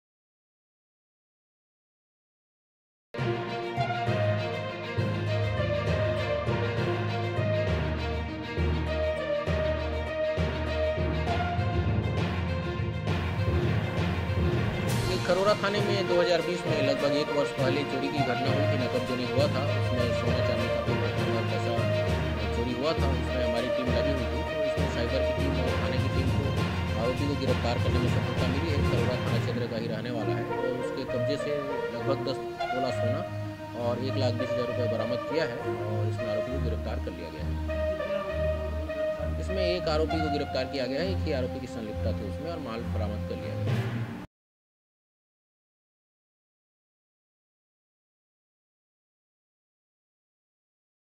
चोरी का खुलासा : शातिर आरोपी को पुलिस ने दबोचा | Newsplus21
चोरी का खुलासा : शातिर आरोपी को पुलिस ने दबोचा

-  खरोरा थाना क्षेत्र के सूने मकान से चोरी करने वाला शातिर चोर गिरफ्तार
-  शातिर आरोपी का नाम मनोज वर्मा है
-  प्रोफेशनल तरीके से घटना को दिया था अंजाम
-  महंगे शौक को पूरा करने के लिए दिया था घटना को अंजाम
-  1 दिसंबर 2020 को दिया था चोरी की घटना को अंजाम